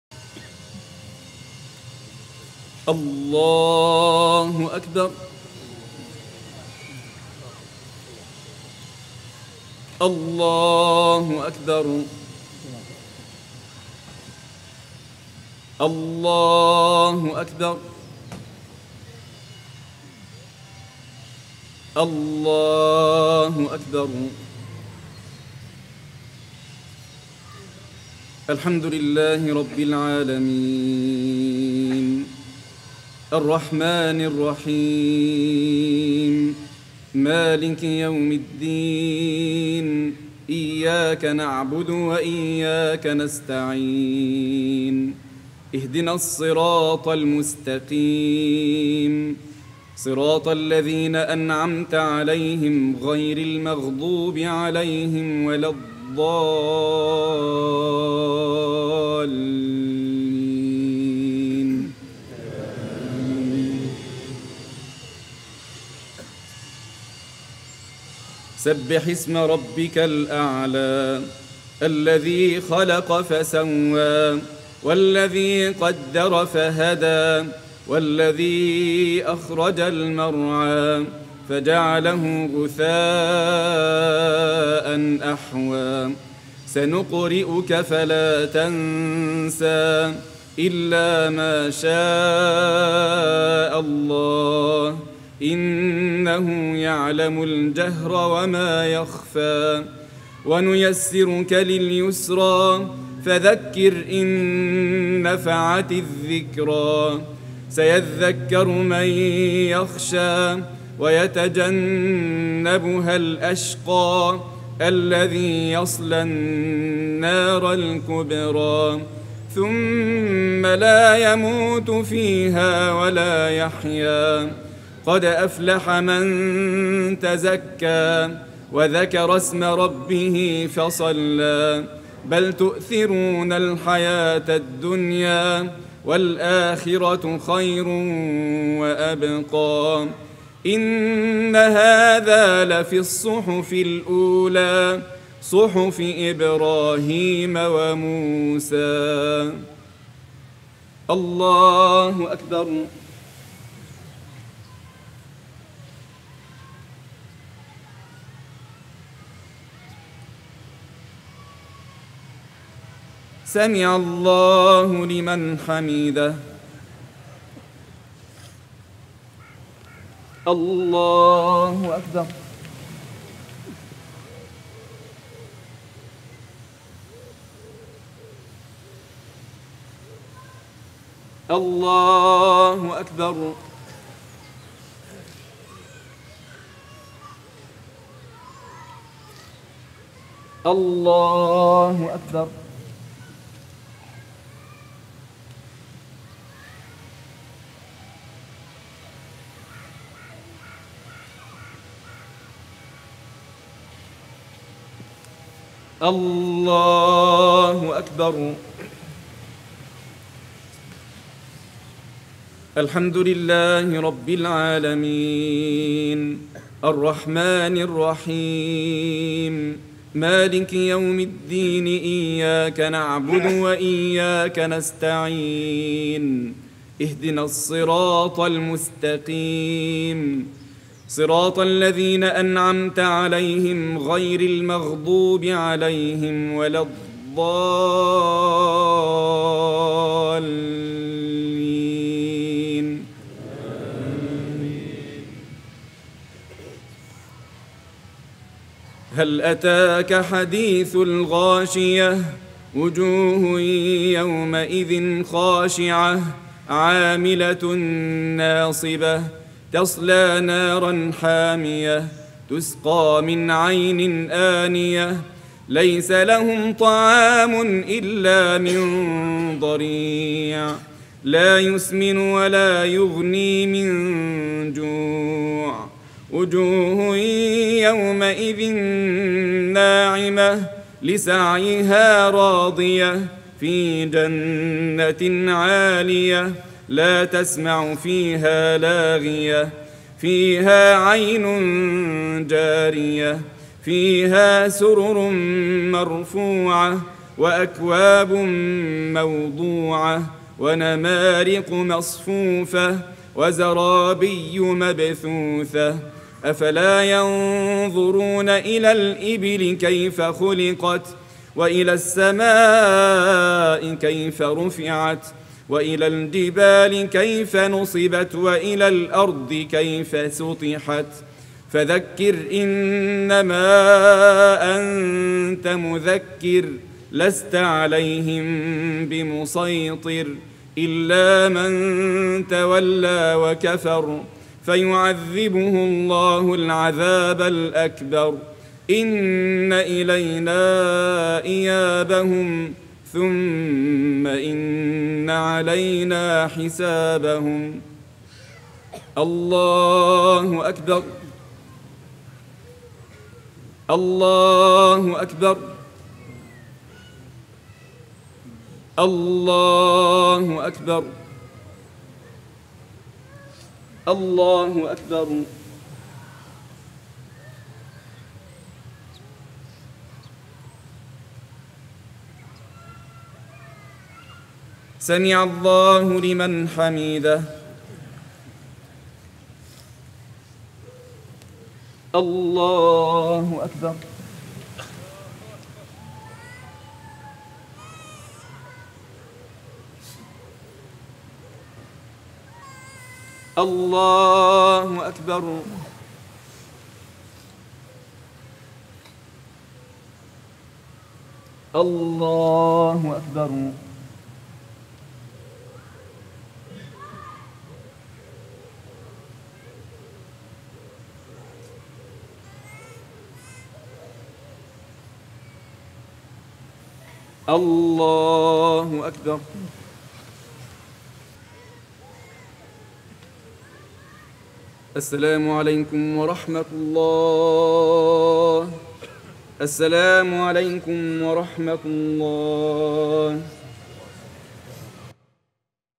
Eid Salaah 2018 in the Park with ArRahma Foundation
Eid ul Adha Salaah 2018 with ArRahma Foundation - Birmingham UK. English translation of the Holy Quran by M. Pickthall